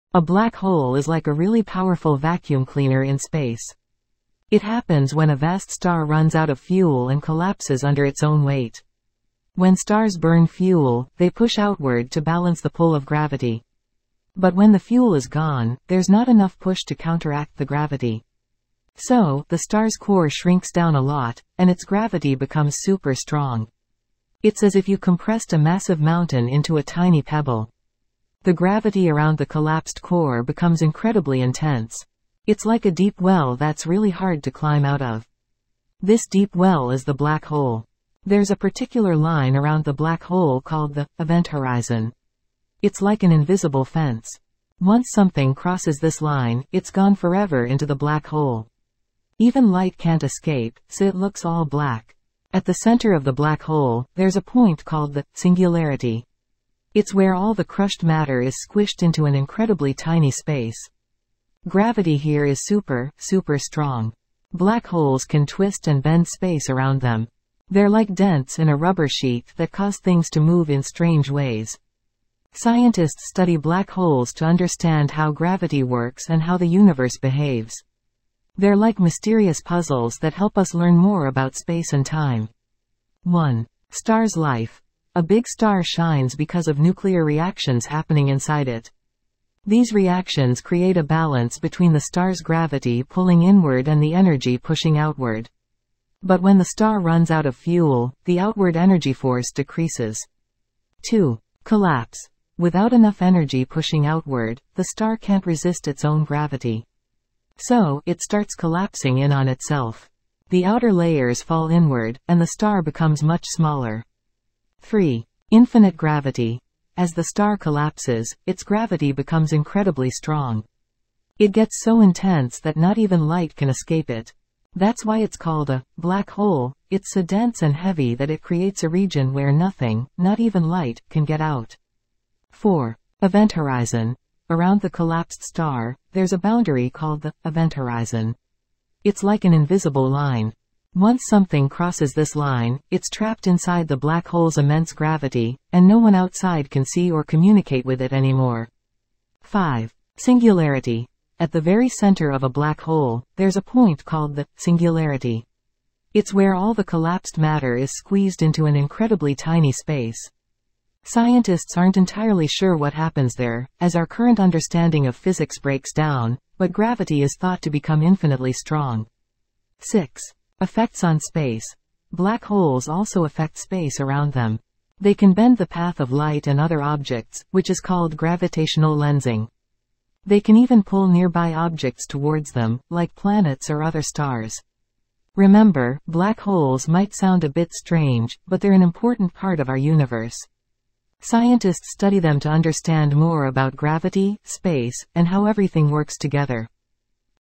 the Black hole
Black Holes
Space Phenomena
Event Horizon
Singularity
Stellar Evolution
Theoretical Physics
A black hole is like a really powerful vacuum cleaner in space. It happens when a vast star runs out of fuel and collapses under its own weight.
When stars burn fuel, they push outward to balance the pull of gravity. But when the fuel is gone, there's not enough push to counteract the gravity.
So, the star's core shrinks down a lot, and its gravity becomes super strong. It's as if you compressed a massive mountain into a tiny pebble.
The gravity around the collapsed core becomes incredibly intense. It's like a deep well that's really hard to climb out of. This deep well is the black hole.
There's a particular line around the black hole called the "event horizon." It's like an invisible fence. Once something crosses this line, it's gone forever into the black hole. Even light can't escape, so it looks all black.
At the centre of the black hole, there's a point called the "singularity." It's where all the crushed matter is squished into an incredibly tiny space. Gravity here is super, super strong.
Black holes can twist and bend space around them. They're like dents in a rubber sheet that cause things to move in strange ways.
Scientists study black holes to understand how gravity works and how the universe behaves. They're like mysterious puzzles that help us learn more about space and time.
1. Star's Life: A big star shines because of nuclear reactions happening inside it. These reactions create a balance between the star's gravity pulling inward and the energy pushing outward. But when the star runs out of fuel, the outward energy force decreases.
2. Collapse: Without enough energy pushing outward, the star can't resist its own gravity. So, it starts collapsing in on itself. The outer layers fall inward, and the star becomes much smaller.
3. Infinite Gravity: As the star collapses, its gravity becomes incredibly strong. It gets so intense that not even light can escape it. That's why it's called a "black hole" - it's so dense and heavy that it creates a region where nothing, not even light, can get out.
4. Event Horizon: Around the collapsed star, there's a boundary called the "event horizon." It's like an invisible line. Once something crosses this line, it's trapped inside the black hole's immense gravity, and no one outside can see or communicate with it anymore.
5. Singularity: At the very centre of a black hole, there's a point called the "singularity." It's where all the collapsed matter is squeezed into an incredibly tiny space. Scientists aren't entirely sure what happens there, as our current understanding of physics breaks down, but gravity is thought to become infinitely strong.
6. Effects on Space: Black holes also affect space around them. They can bend the path of light and other objects, which is called gravitational lensing. They can even pull nearby objects towards them, like planets or other stars.
Remember, black holes might sound a bit strange, but they're an important part of our universe. Scientists study them to understand more about gravity, space, and how everything works together.